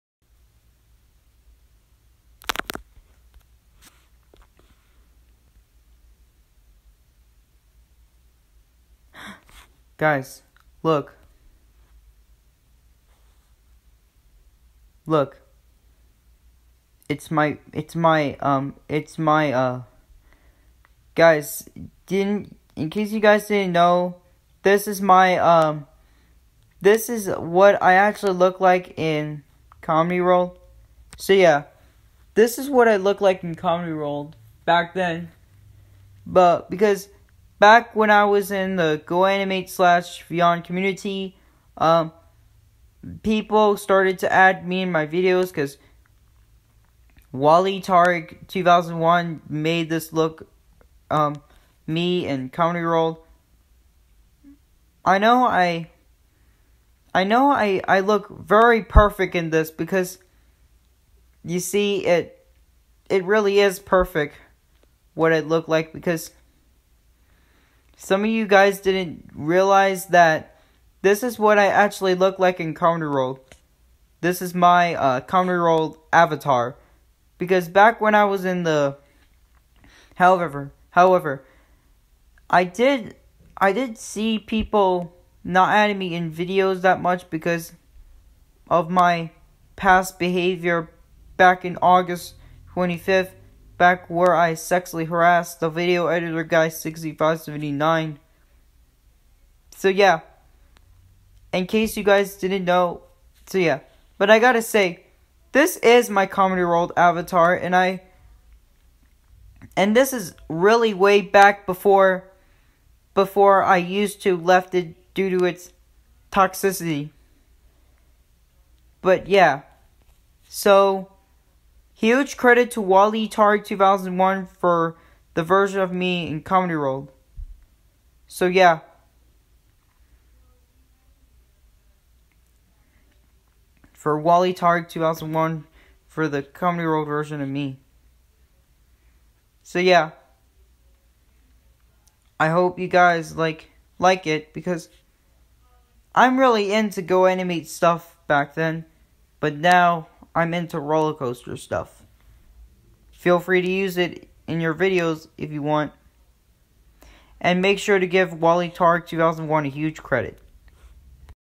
I saw my Comdey World Avatar!
When I was on Google Docs, I saw a picture of my old avatar of me back when you guys add me in Comdey World when I was used to be “Narman Studios” or “NarmanStudiosYTAnimate2006//NarmanTunes2006”! By the way! Credit to @Waleed Tariq2001 for the Comdey World version of me!

And don’t forget to send me requests for a new video!
-COASTER BOT